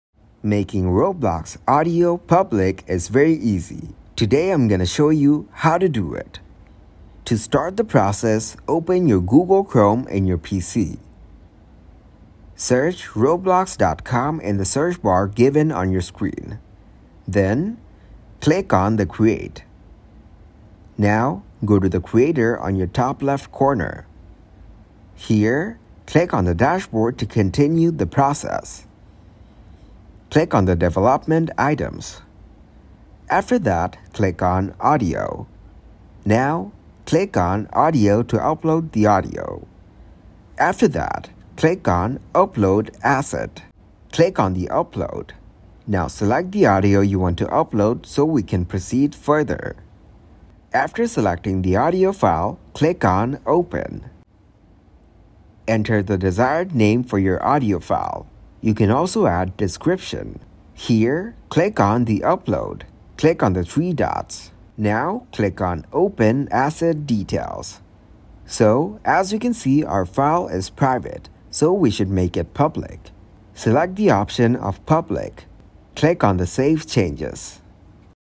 HOW TO MAKE ROBLOX AUDIO PUBLIC 2023! (BEST WAY) 2024
In this video, we will walk you through the step-by-step process of How To Make roblox Audio Public. We'll delve into the details, provide valuable insights, and ensure you have all the information you need to succeed. Here's what you can expect:

0:00 INTRO
0:06 TUTORIAL